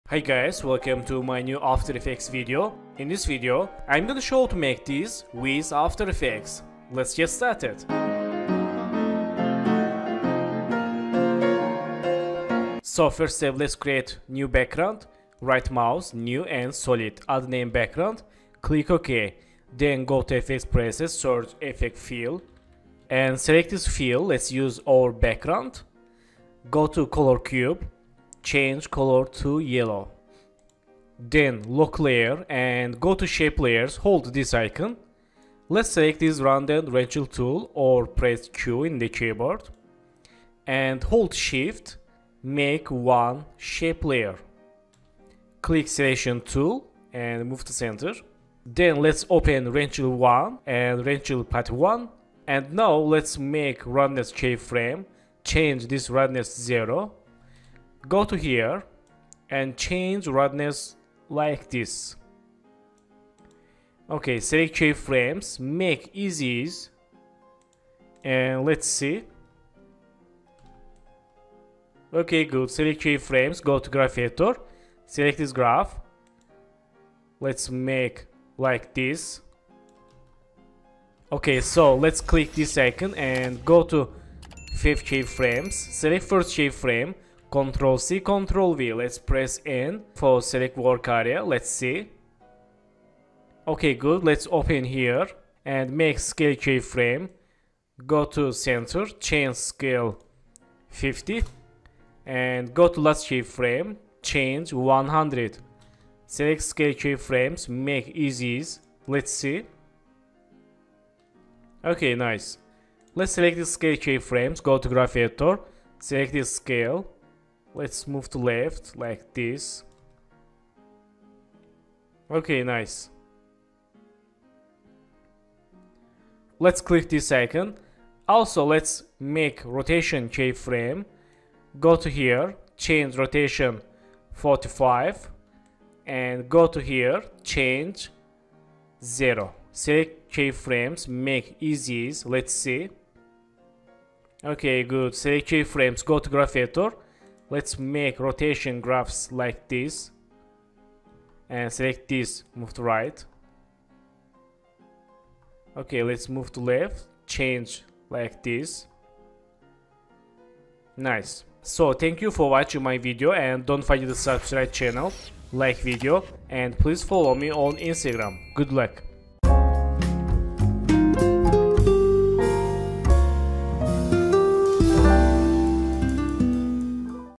Circle to Rectangle - After Effects Shape Morping Tutorials
Hi guys, welcome to my new After Effects tutorial. In this tutorial i will show how to circle morphing to rectangle in After Effects shape layer tutorials. Let's get started!